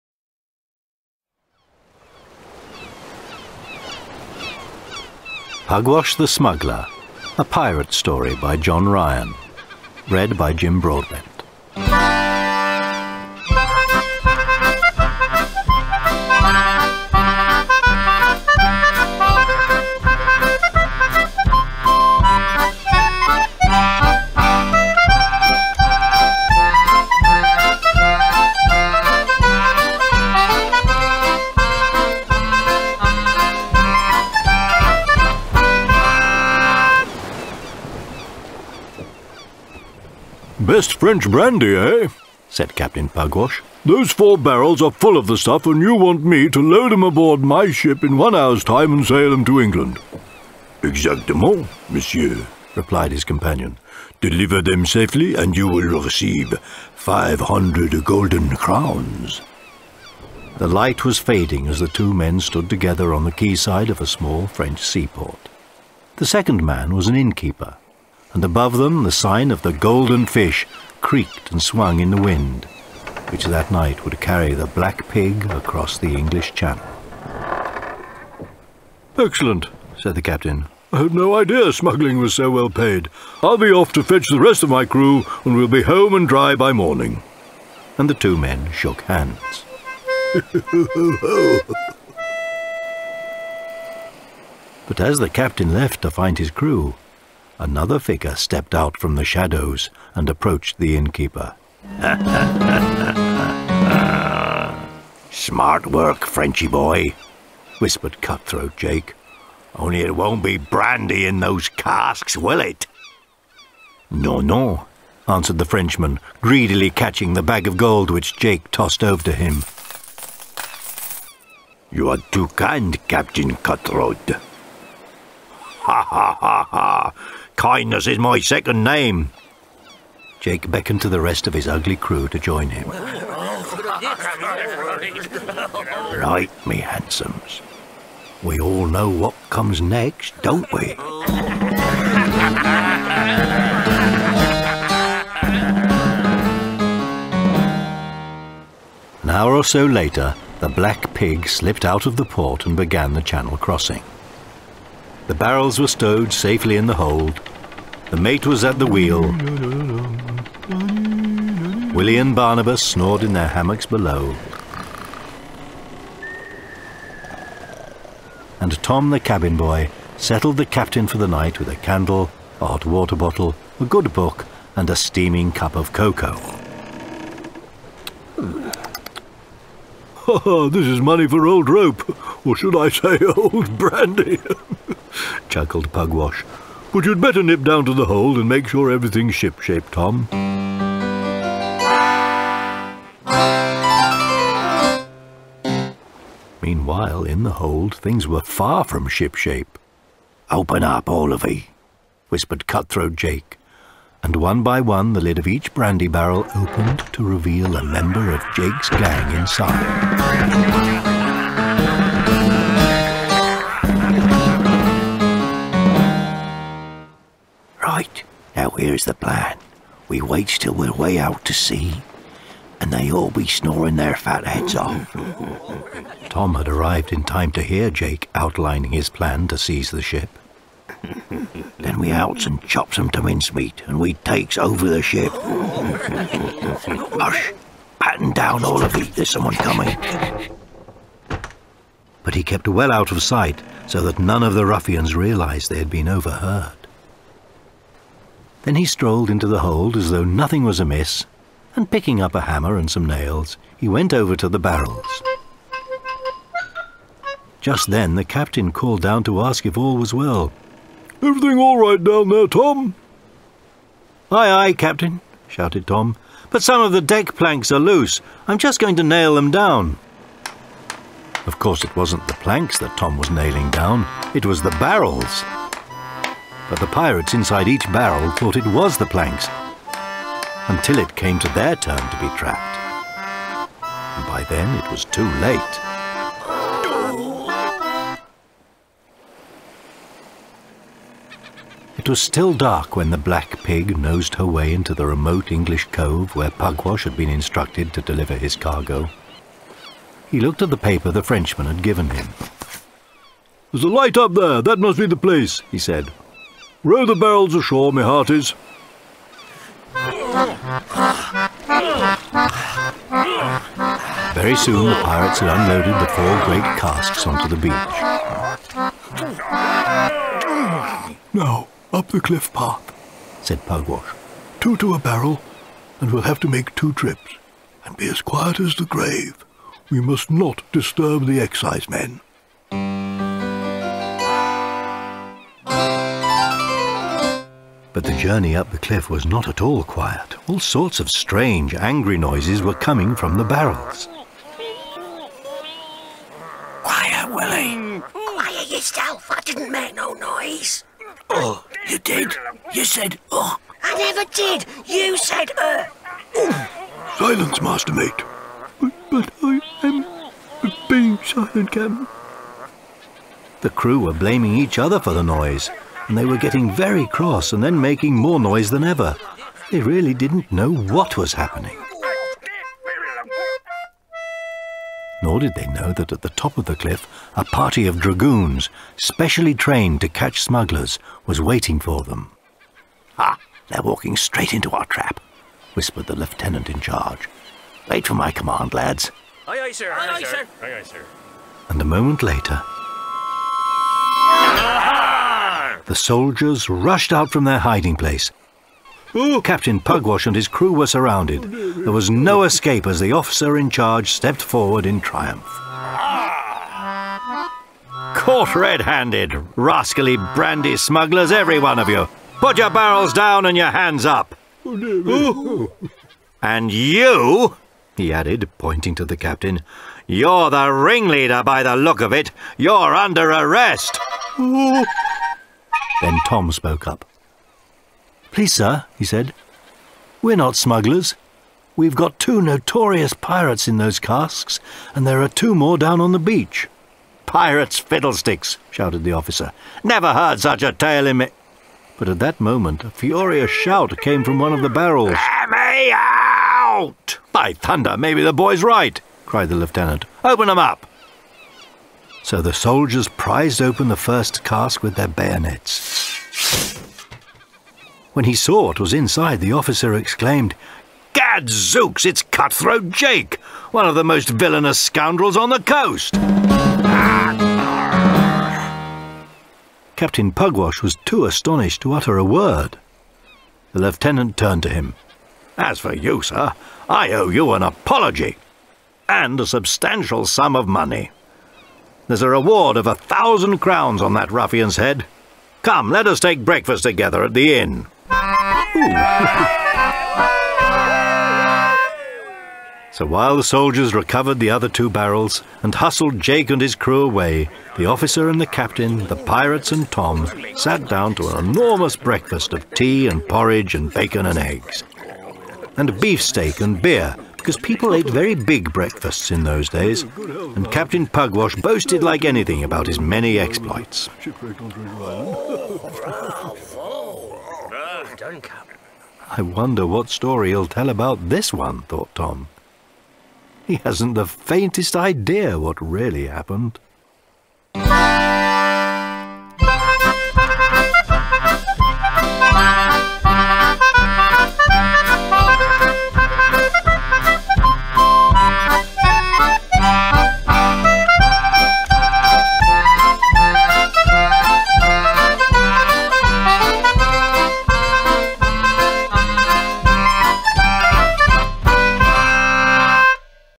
Pugwash the Smuggler Story (Audio Book Version)
This is the Audio Book Version of Pugwash the Smuggler and it's read By Jim Broadbent.